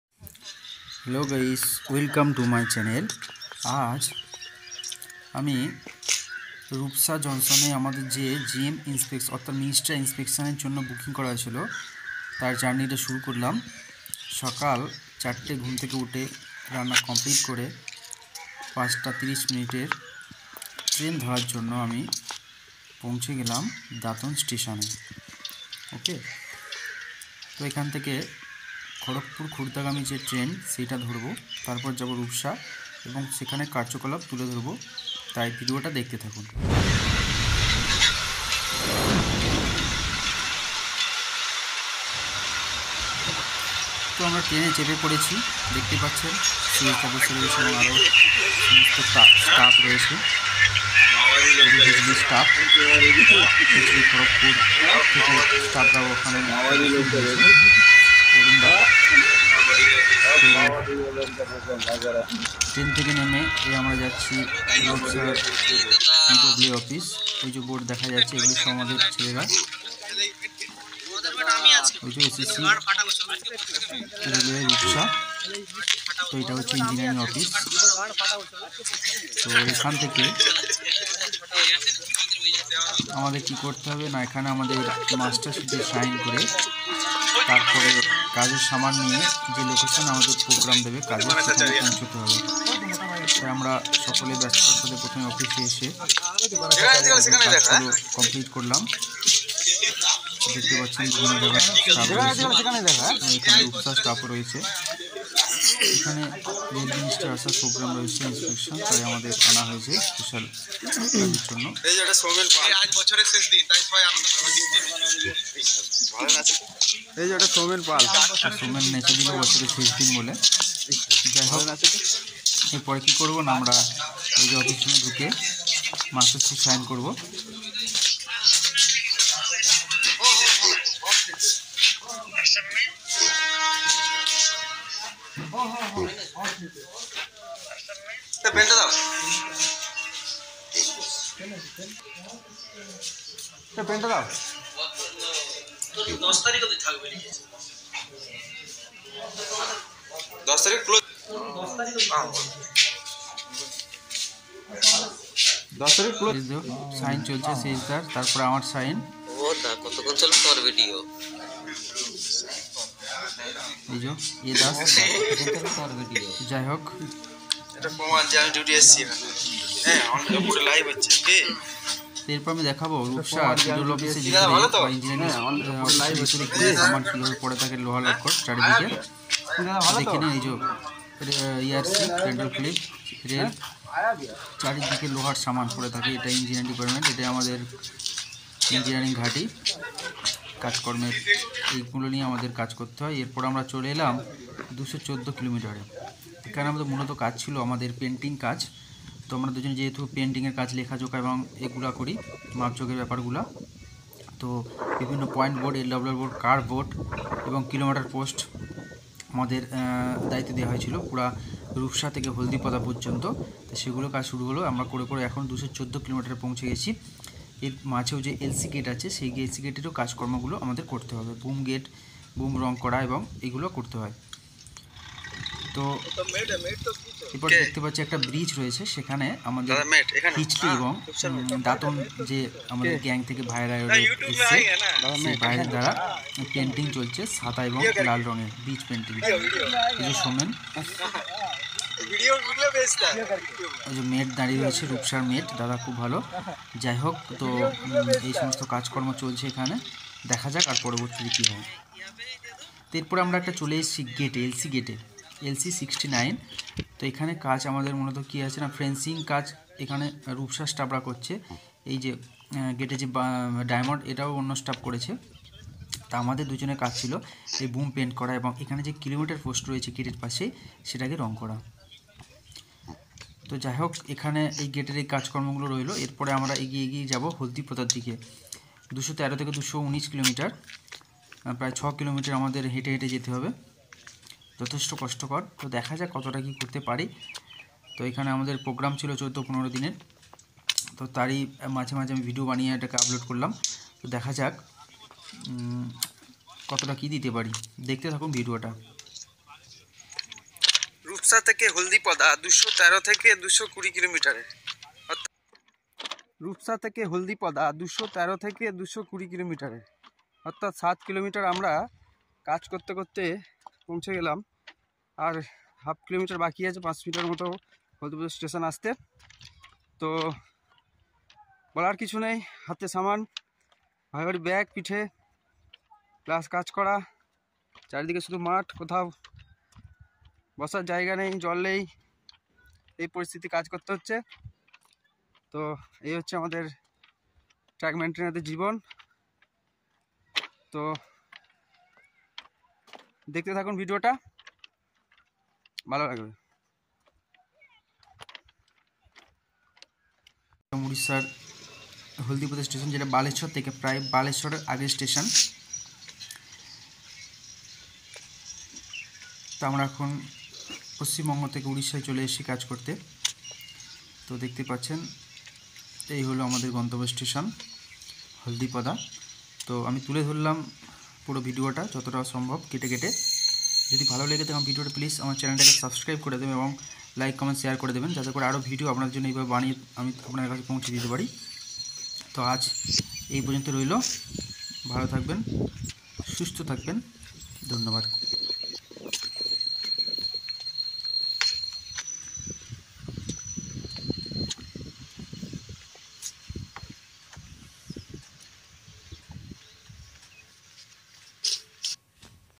Rupsa Junction Minister inspection programme work//Painting work at Rupsa.//Trackman work..
Rupsa  JunctionMinister inspection programme//Painting work at Rupsa.//Trackman work..

we are under sse/pway/don but booked for Minister inspection programme at Rupsa junction.

specially for painting work like.

   gate boom painting,gate post painting,diamond making,curve board,LWR board,Point board,bridge paint,km post paint...